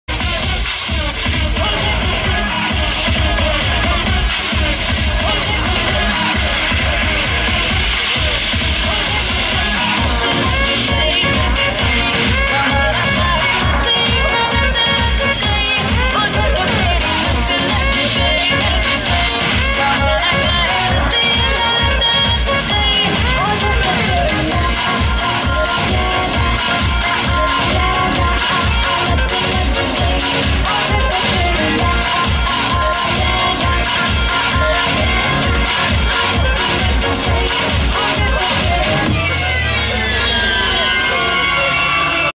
Desi Bravehearts
Desi Bravehearts on the Commonwealth Stage at O2 Glasgow Mela 2013